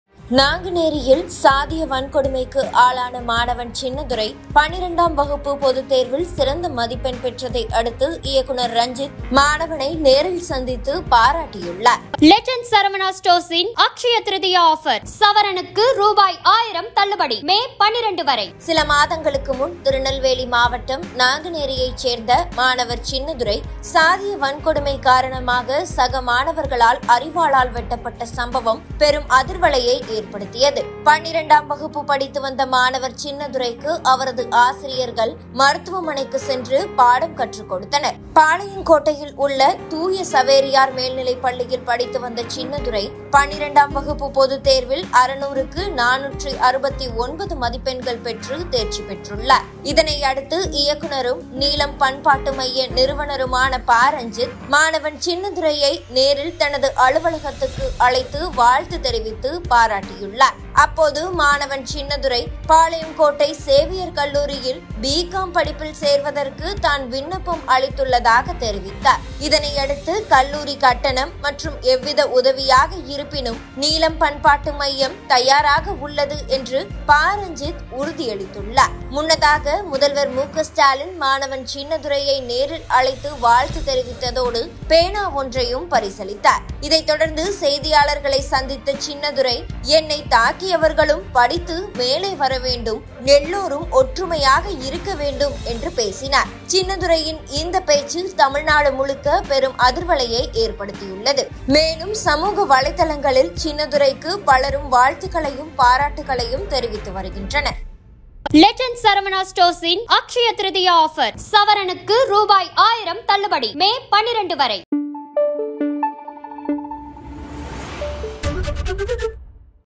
Pa Ranjith wish Nanguneri Chinnadurai|சின்னதுரைக்கு பரிசு வழங்கிய பா.ரஞ்சித்!நேரில் அழைத்து பாராட்டு
Pa Ranjith wish Nanguneri Chinnadurai|சின்னதுரைக்கு பரிசு வழங்கிய பா.ரஞ்சித்!நேரில் அழைத்து பாராட்டு | Neelam | MK Stalin | 12th Exam Results | Tamil News

"அட்சய திருதியை OFFER – மே 3 முதல் 12 வரை

தங்க நகைகள் பவுனுக்கு ரூ.1000 தள்ளுபடி

THE LEGEND NEW SARAVANA STORES, PADI"